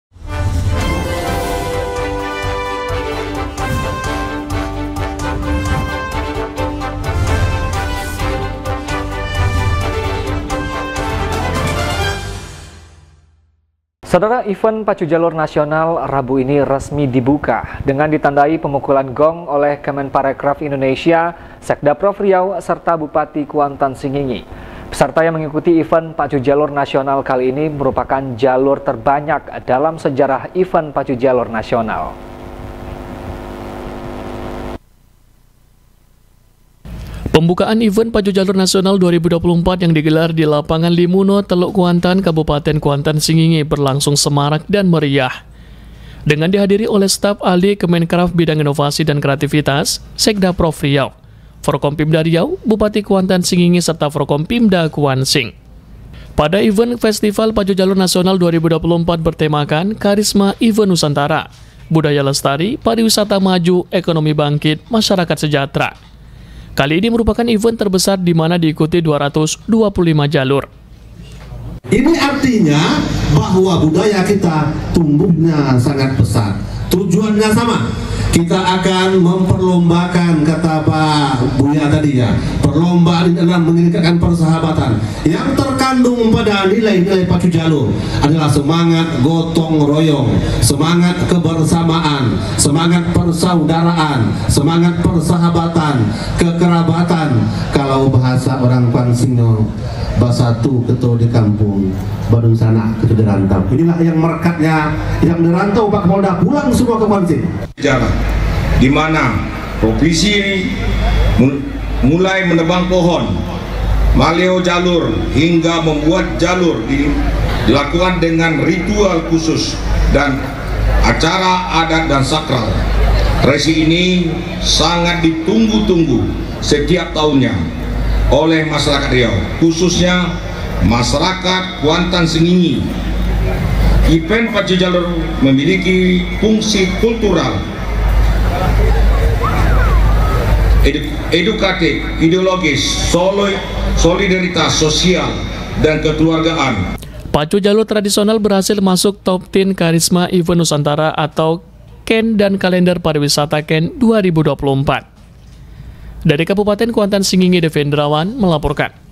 EVENT PACU JALUR NASIONAL 2024 RESMI DIGELAR
Event Pacu Jalur Nasional Rabu ini resmi dibuka dengan ditandai pemukulan gong oleh Kemenparekraf Indonesia, Sekdaprov Riau serta Bupati Kuantan Singingi. Peserta yang mengikuti event Pacu Jalur Nasional kali ini merupakan jalur terbanyak dalam sejarah event Pacu Jalur Nasional.